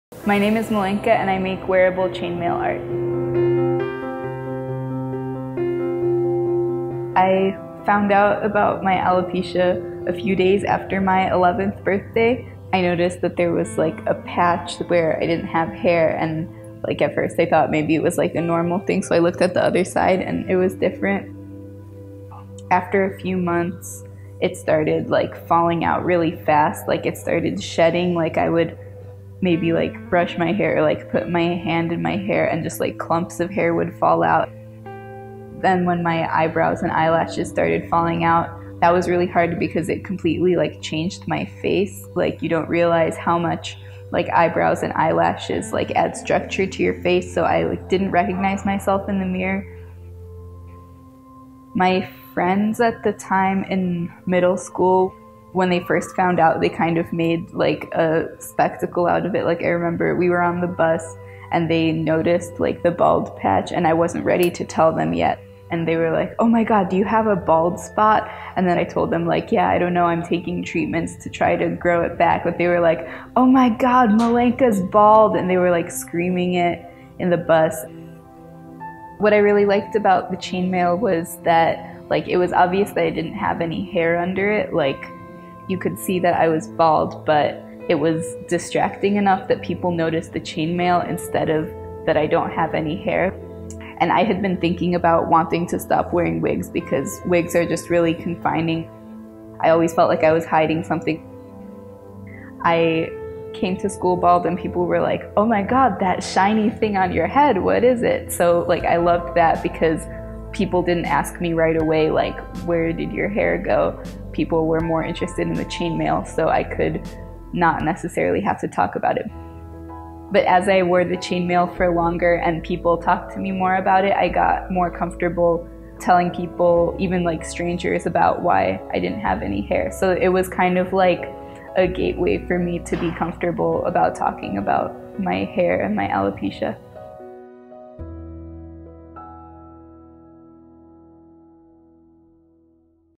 Milenka Bermanova: Chainmail Artist
At age 11, Milenka Bermanova found out they had alopecia.  They started wearing wigs, but found it confining and extremely uncomfortable. In high school, Milenka started making one-of-a- kind chain mail pieces to cover their head in lieu of wigs.

🎥: superpaulinator